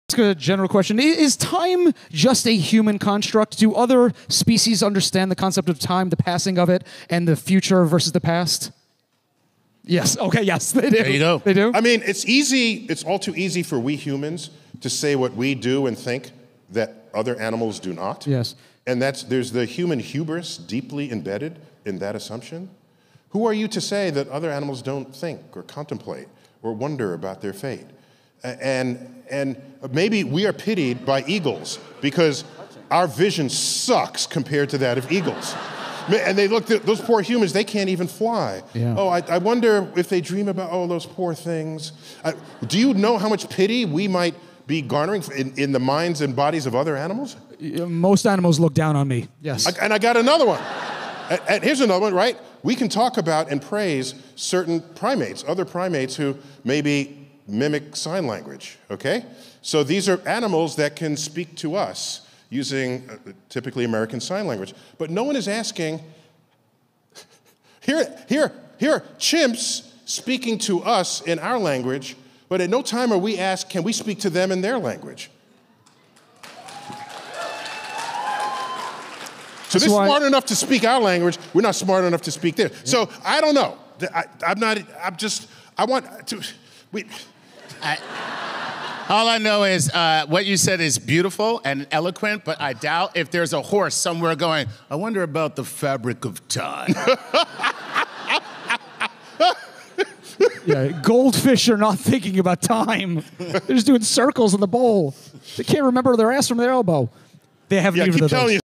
Is time just a human construct explained by brian greene and neil degrasse tyson
The concept of MEASURING time is a human construct. So hours, minutes, seconds, etc will cease to exist as measurements, but the passage of time will still go on.